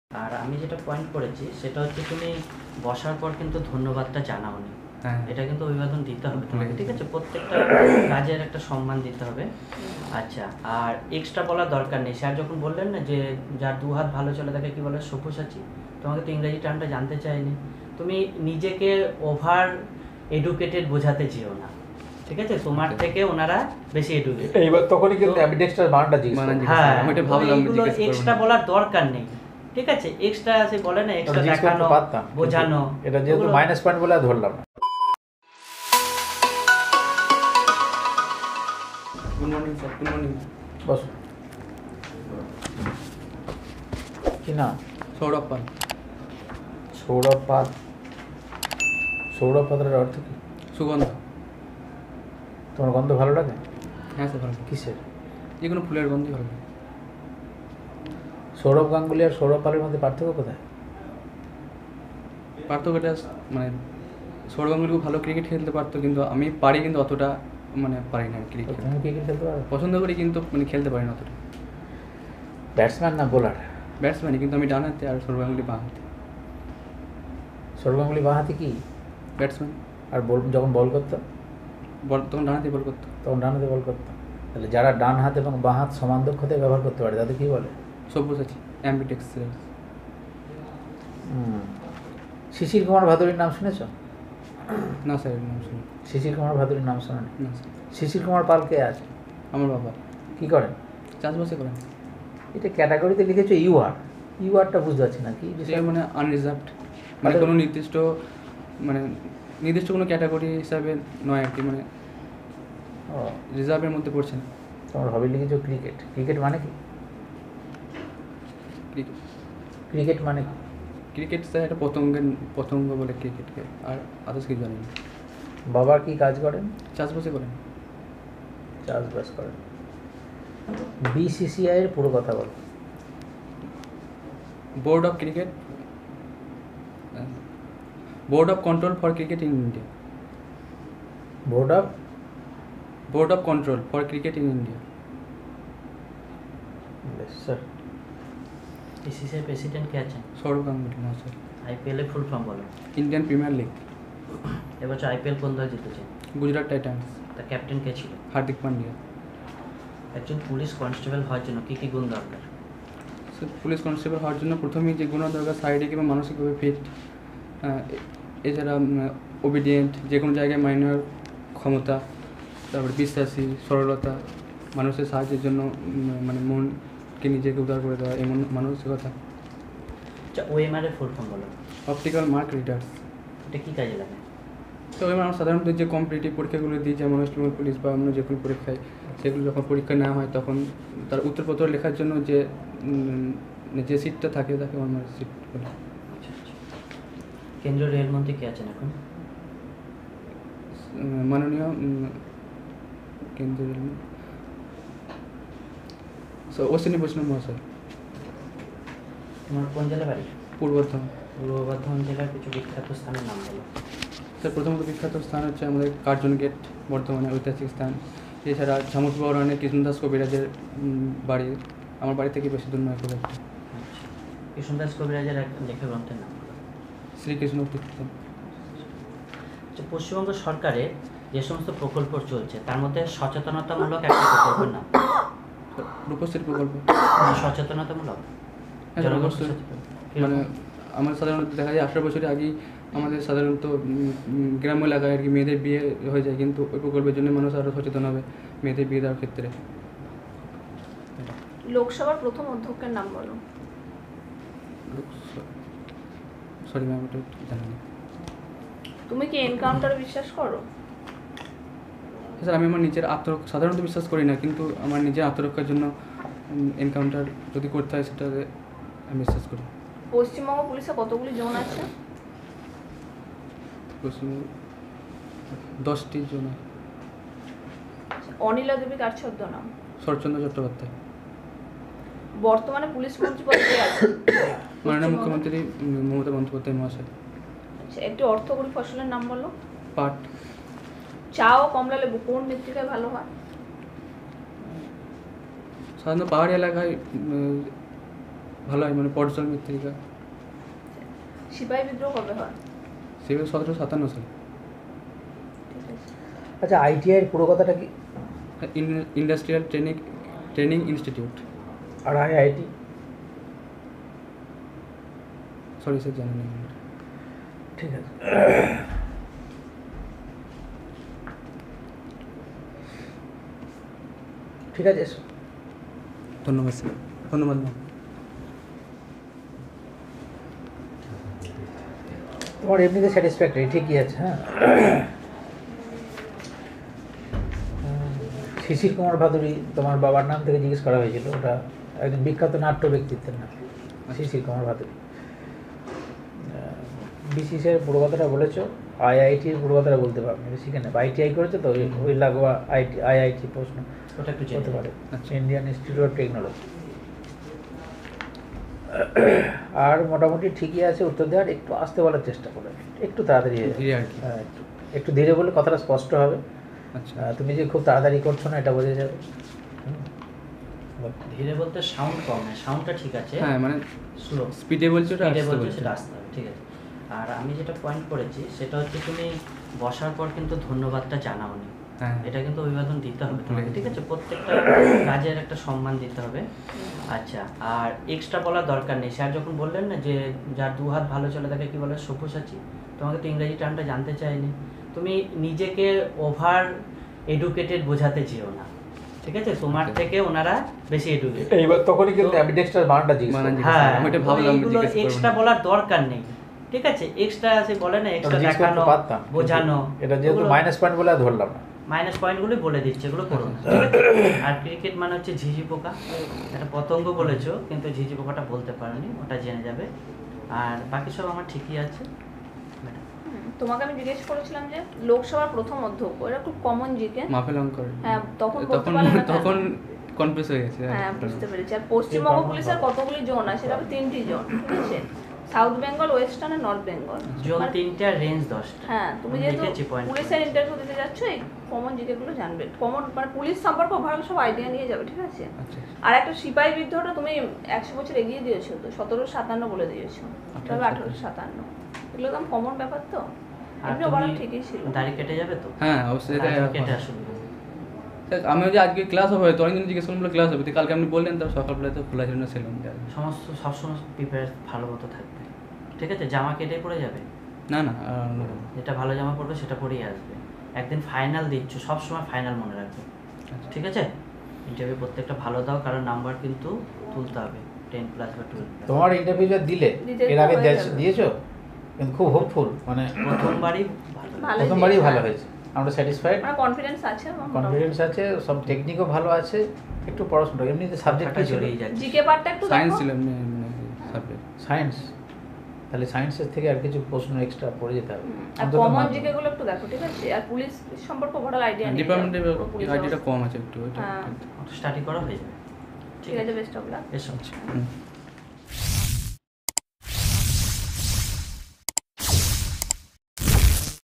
WBP Constable Mock Interview 2022║পশ্চিমবঙ্গ পুলিশ ইন্টারভিউ║Interview & Review║Mock no - 02
This video was made for the WBP Constable Interview and to point out the mistakes of the students.

পশ্চিমবঙ্গ পুলিশ -এর ইন্টারভিউ ও রিভিউ গুলি আমাদের চ্যানেলে আপলোড করা হয়েছে। আশা করছি তোমাদের ভালো লাগবে। Thanks🥰